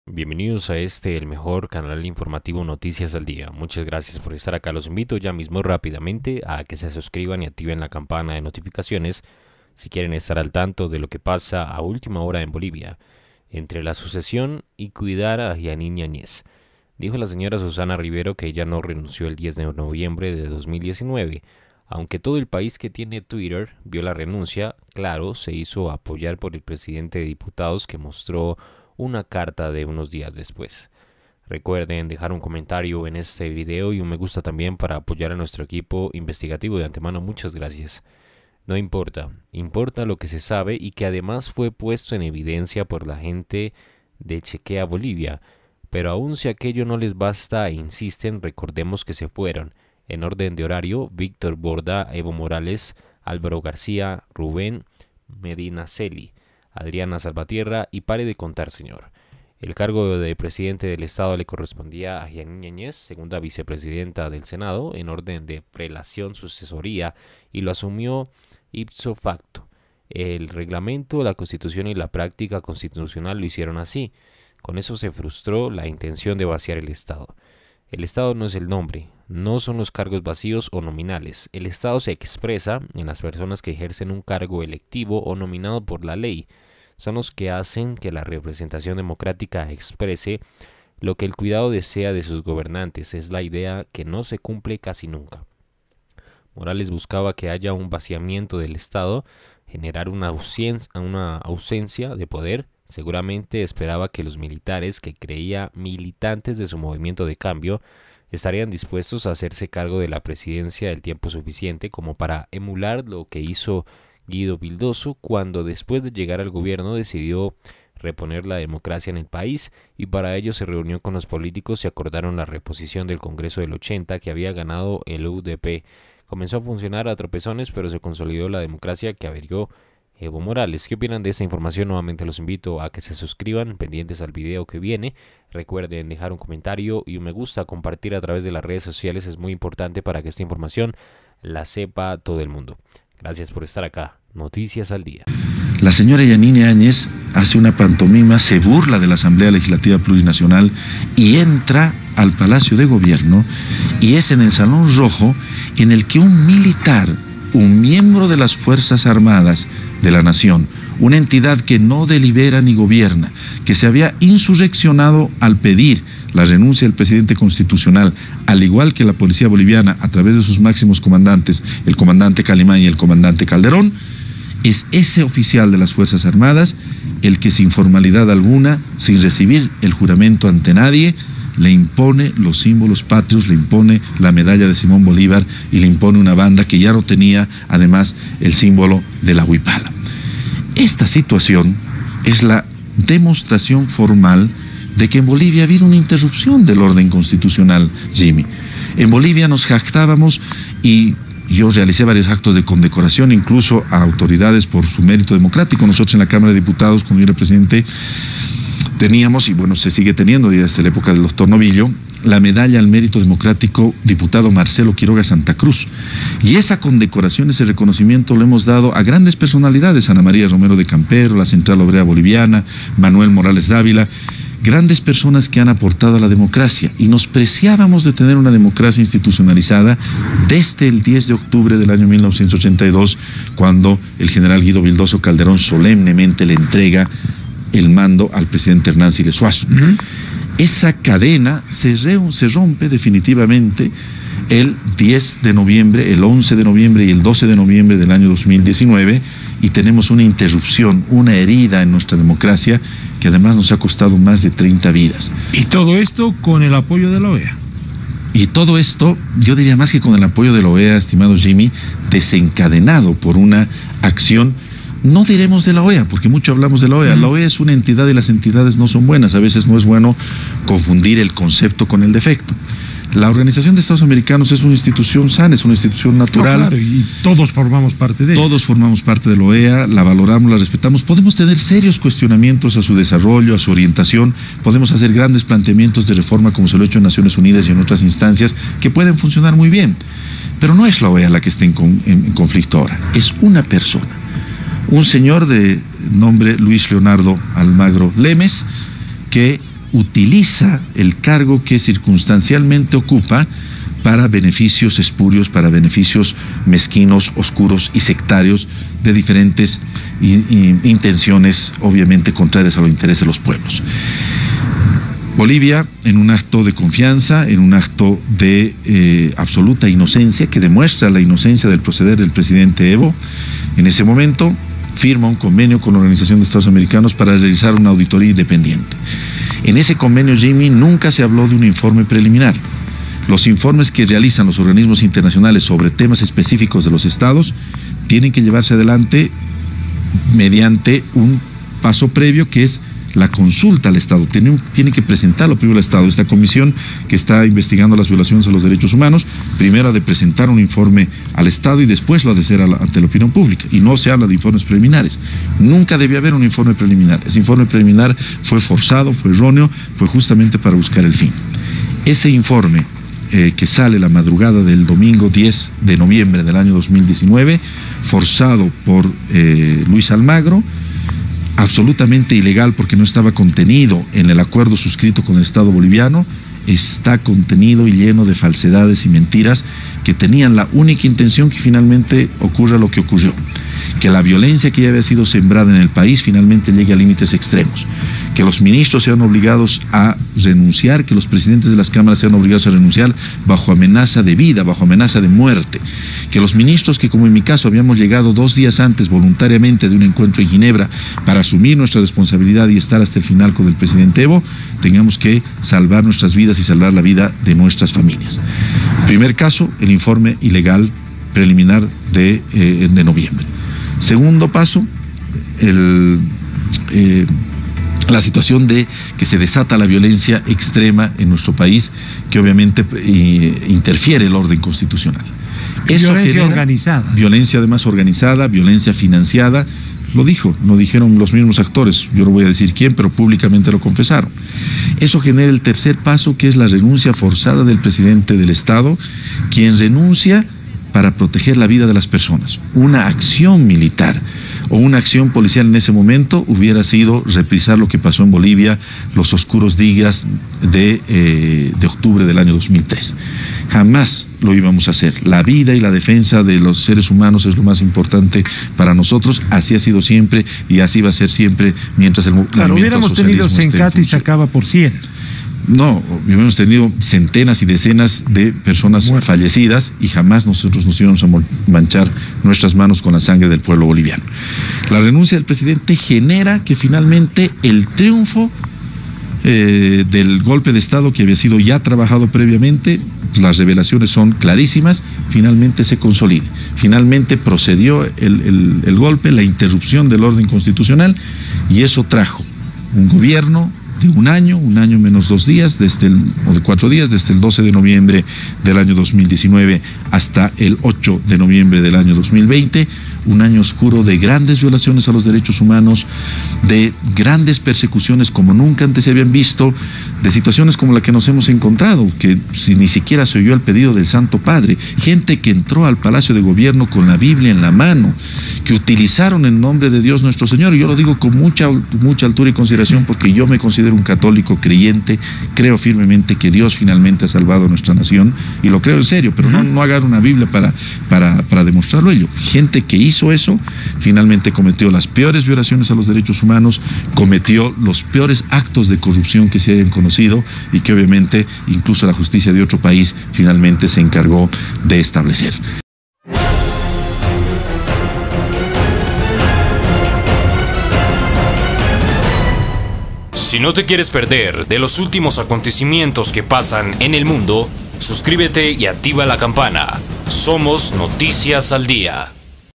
BOLIVIA HOY NOTICIAS 27 DE JUNIO 2021, PRONTA SALIDA DE JEANINE AÑEZ? ULTIMA HORA SEGUROS HOY
Gracias!

ultima hora bolivia hoy 27 de junio 2021
noticias de bolivia hoy 27 de junio 2021